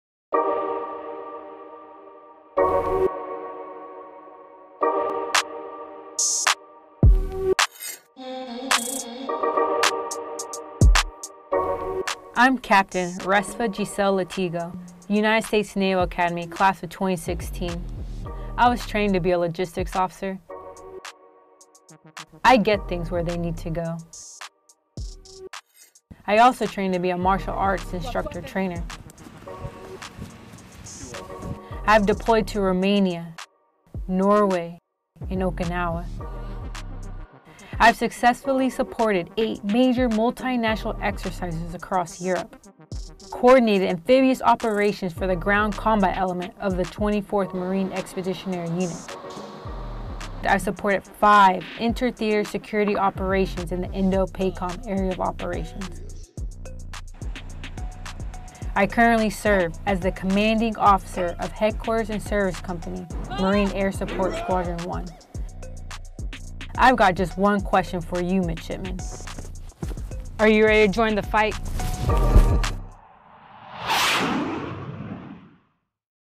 Capt. Resfa Latigo addresses U.S. Naval Academy Midshipmen
MARINE CORPS AIR STATION CHERRY POINT, NC, UNITED STATES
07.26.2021

U.S. Marine Corps Capt. Resfa G. Latigo, a logistics officer with Marine Air Support Squadron (MASS) 1, addresses midshipmen from the U.S. Naval Academy, at Marine Corps Air Station Cherry Point, North Carolina, July 28, 2021. Latigo graduated the Naval Academy in 2016. MASS-1 is a subordinate unit of 2nd Marine Aircraft Wing, the aviation combat element of II Marine Expeditionary Force. (U.S. Marine Corps video by Cpl. Damaris Arias)

Film Credits: Video by Cpl. Damaris Arias
2nd Marine Aircraft Wing

USMC
Naval Academy
2nd MAW
MASS-1
Capt Latigo